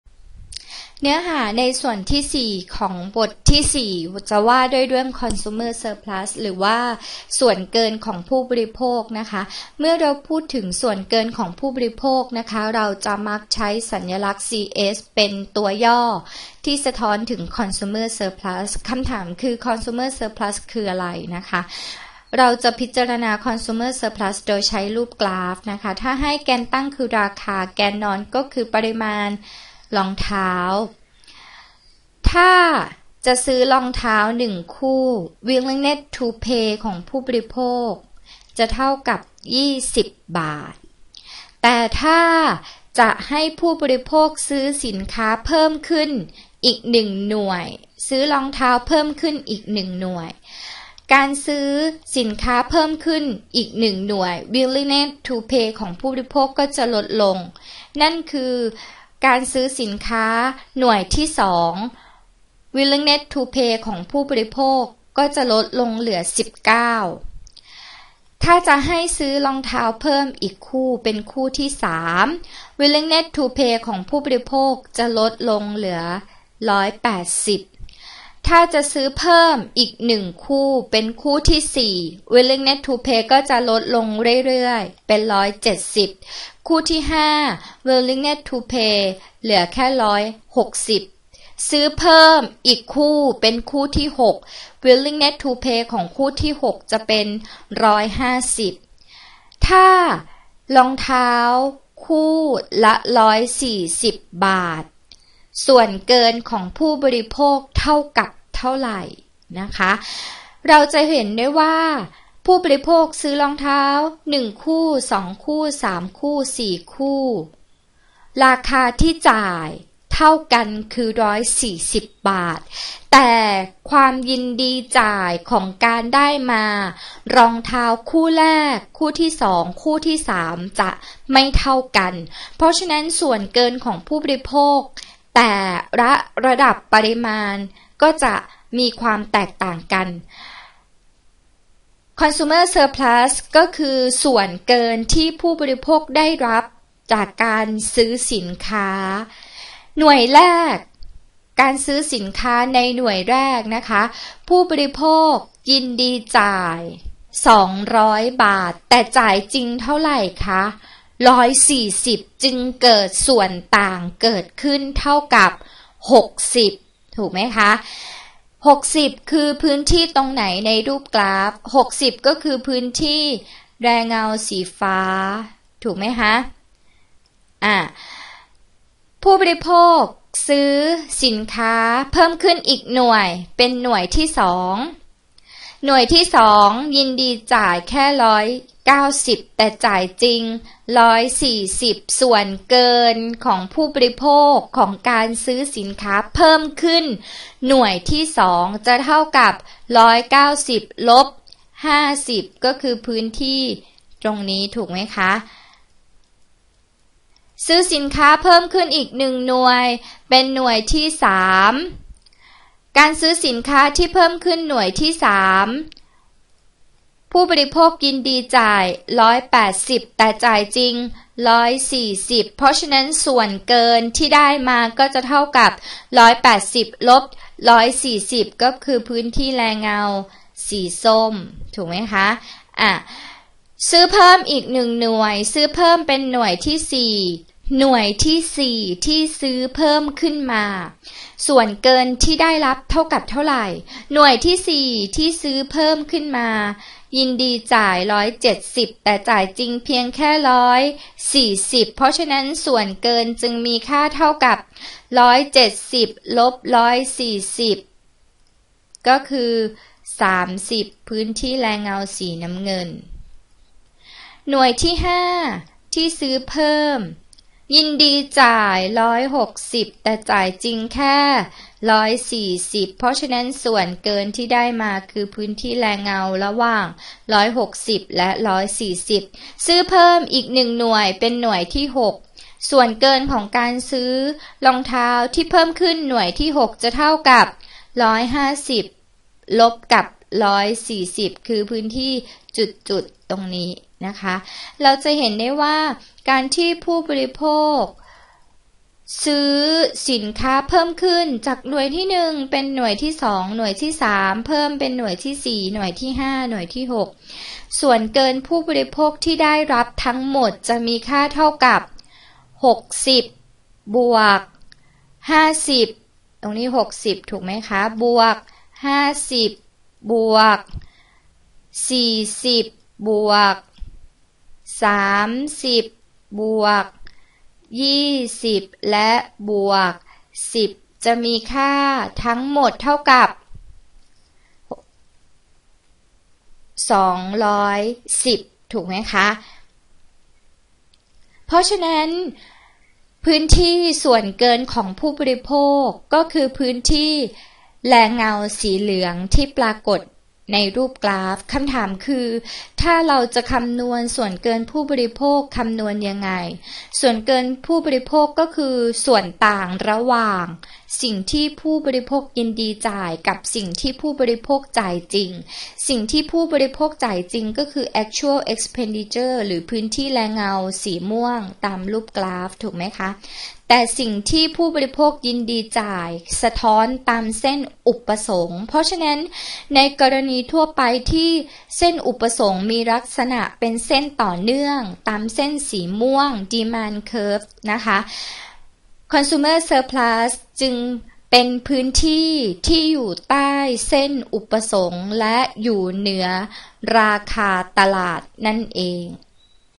875-205: ch.4 ตอนที่ 7 เรื่อง Consumer Surplus
วิชา 875-205 Microeconomics II
จัดทำโดย อ.จริยภัทร รัตโณภาส บุญมา
คณะเศรษฐศาสตร์ ม.สงขลานครินทร์